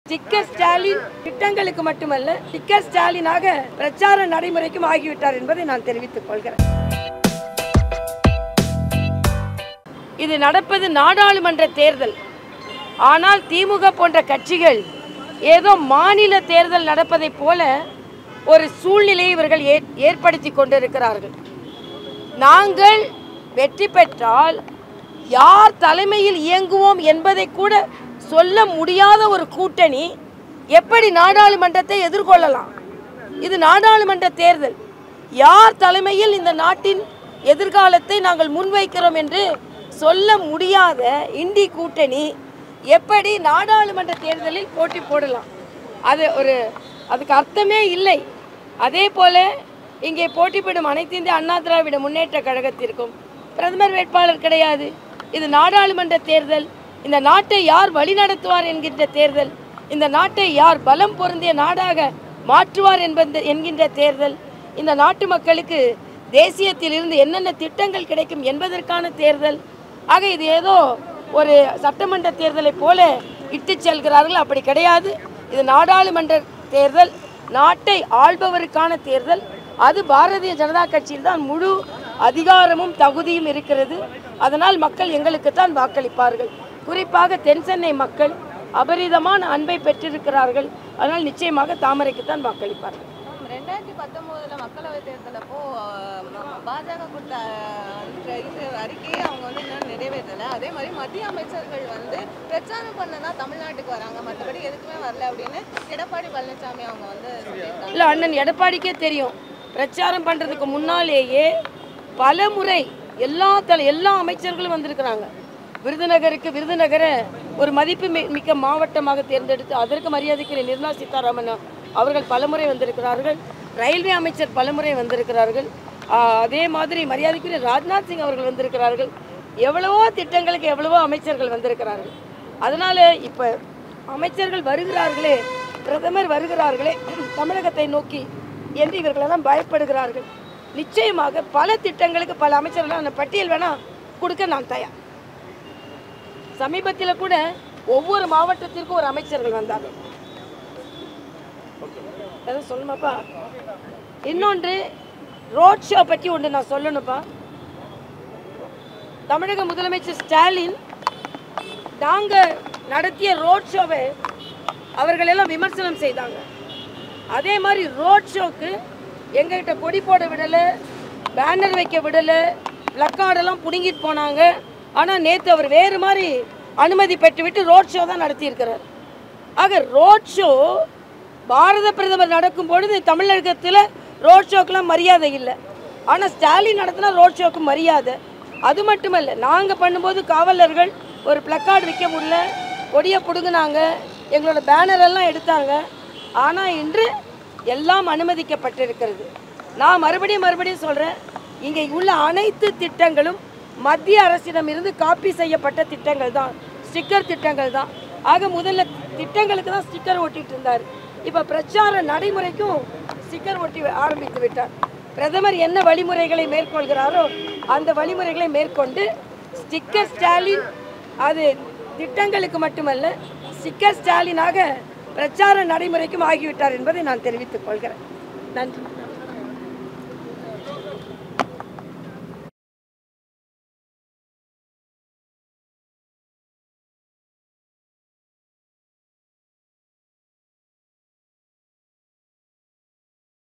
பிரச்சாரத்திற்கும் ஸ்டிக்கர் ஓட்டுகிறார் ஸ்டாலின்| Tamilisai Soundararajan Press Meet | CM MK Stalin
பிரச்சாரத்திற்கும் ஸ்டிக்கர் ஓட்டுகிறார் ஸ்டாலின் | Tamilisai Soundararajan Today Press Meet | CM MK Stalin | CHENNAI EXPRESS